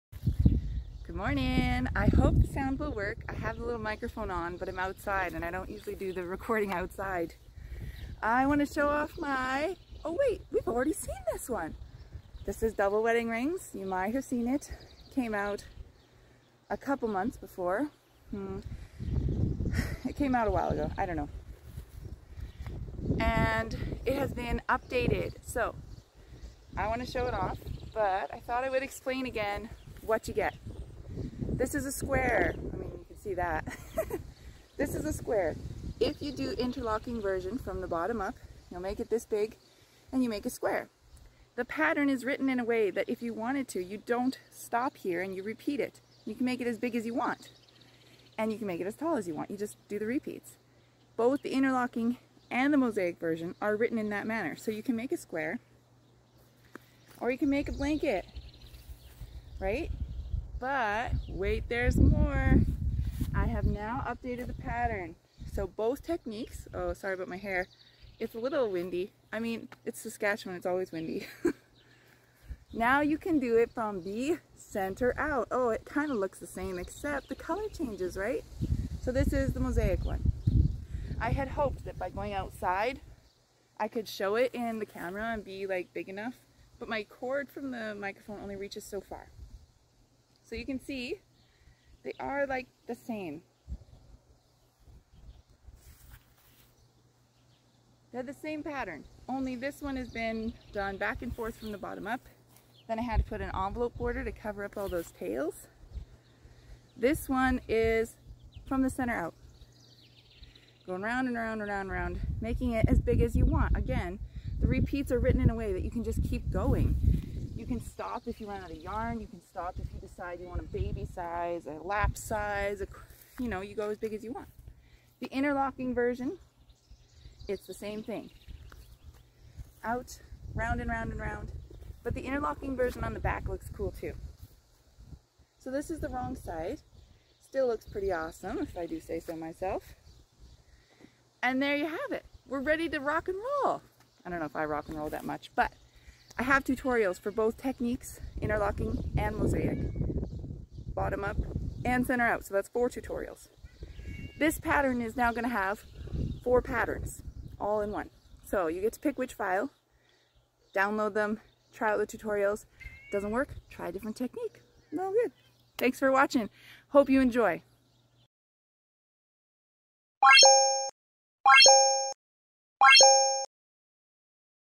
Double Wedding Rings promo video
My most popular pattern (Double Wedding Rings) has now been updated to include center-out instructions for both techniques (interlocking crochet and overlay mosaic crochet)!

or Etsy